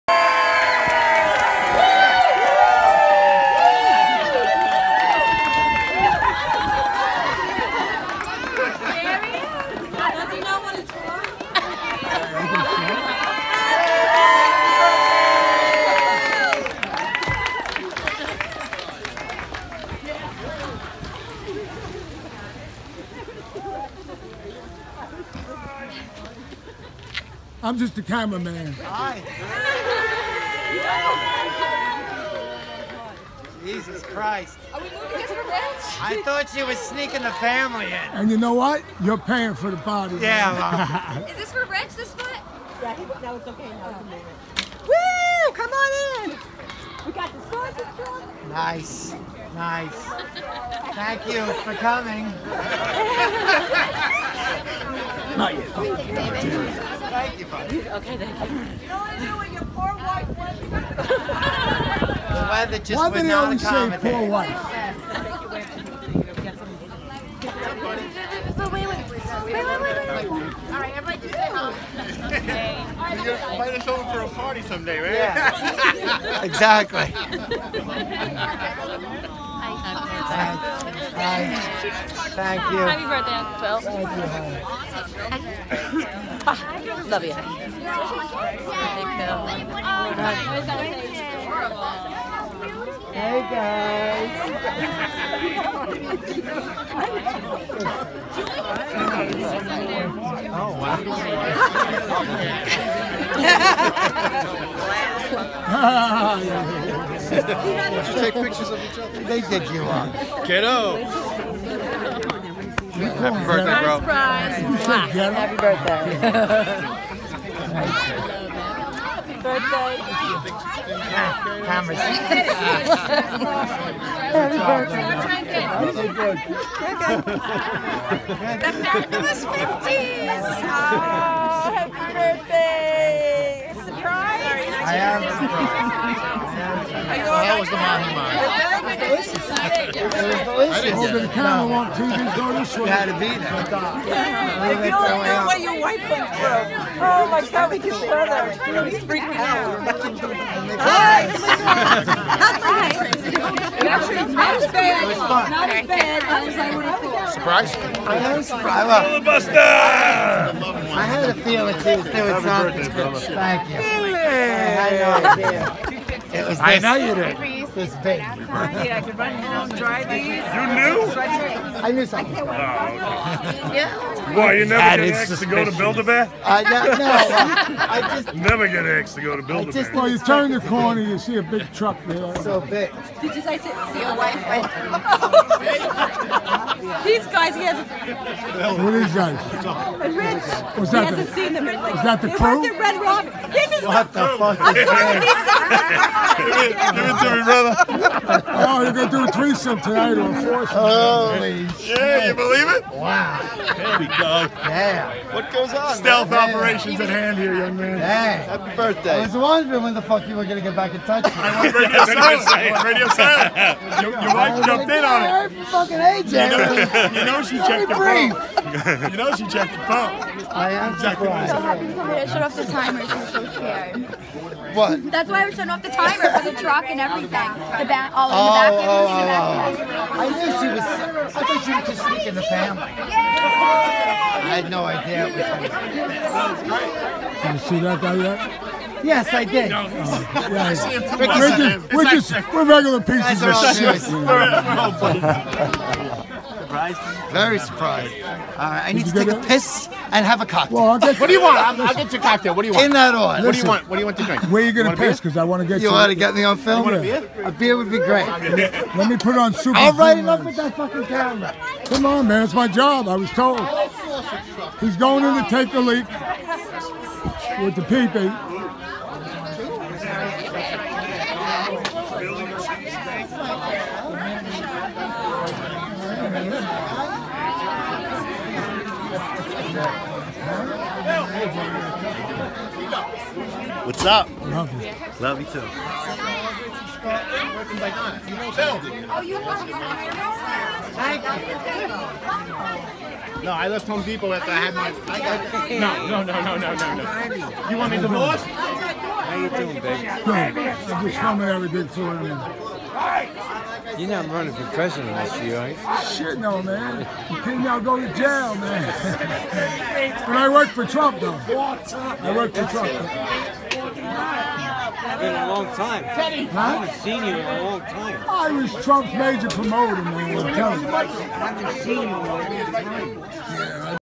Happy 50th Birthday Phil
Phil McGinty gets surprised for his 50th birthday by family and friends